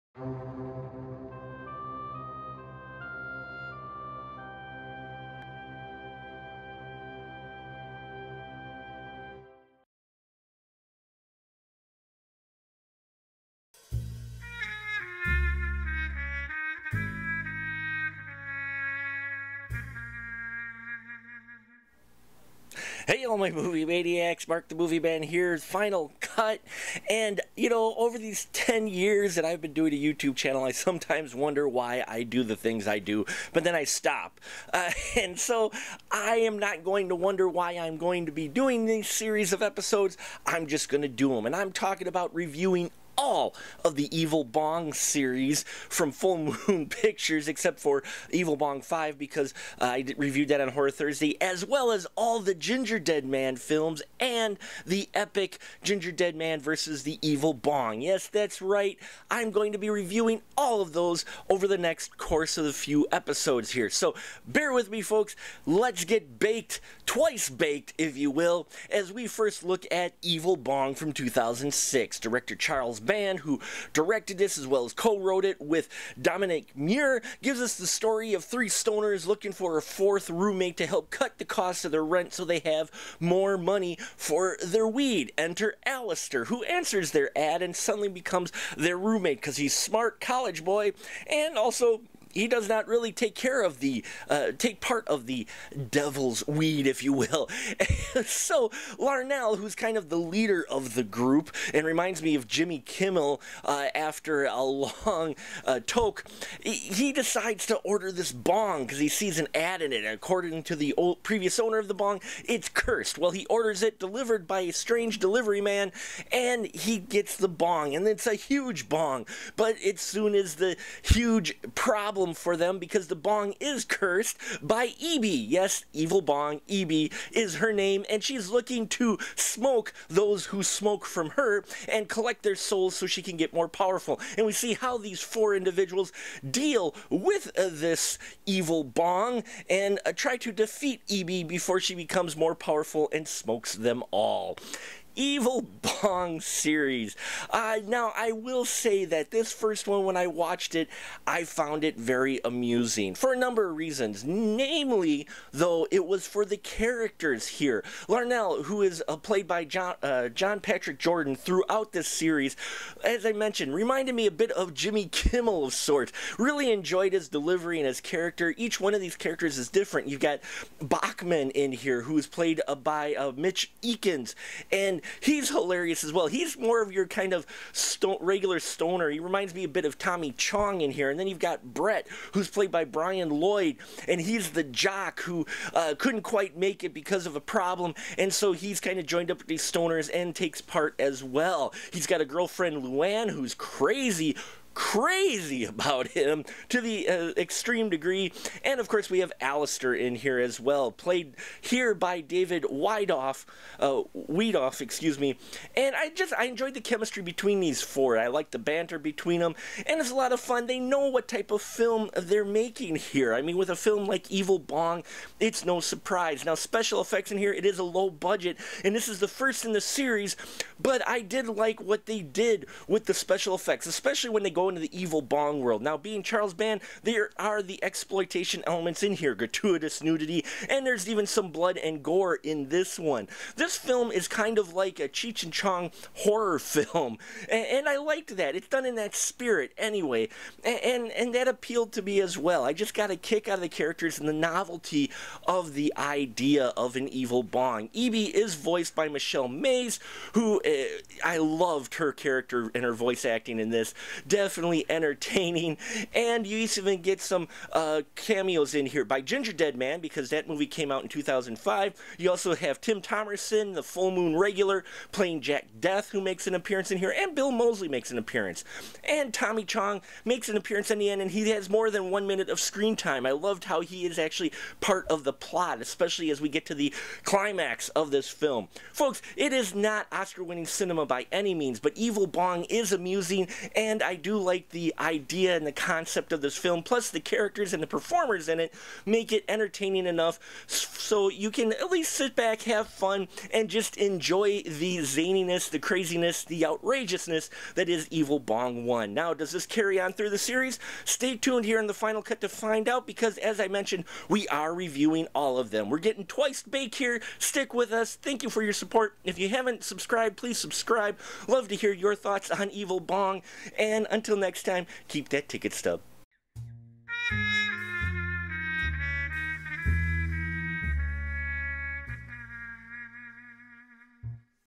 Evil Bong Movie Review
Welcome fellow moviemaniacs! Come with us as we get baked twice with our next series of movie reviews. I call it our "Twice Baked" review series. We will be looking at all the films in the Evil Bong series and the Gingerdead Man series. So prepare yourself accordingly dude and dudettes.